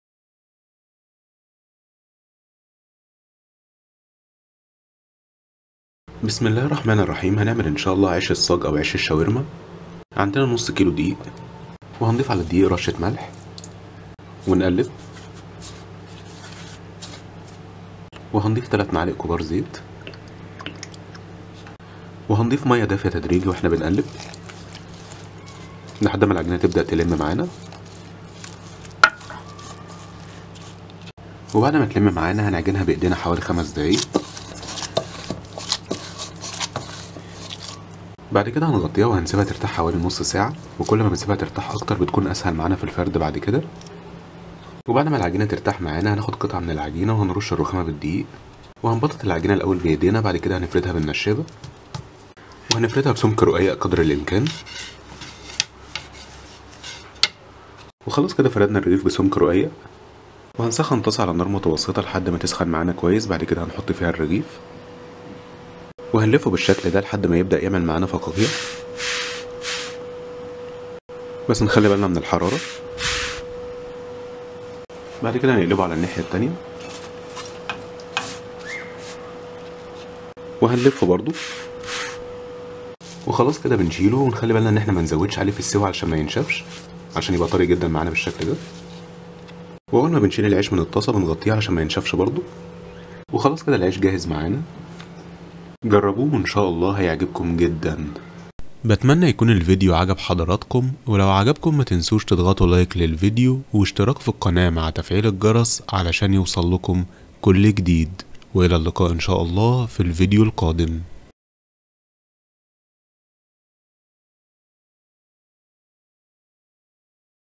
طريقة عمل عيش الشاورما الطري في البيت - خبز الصاج
طريقة عمل عيش الشاورما الطري لعمل افضل ساندويتشات شاورما لحمة او فراخ في البيت ، وصفة خاصة من سنة أولى مطبخ .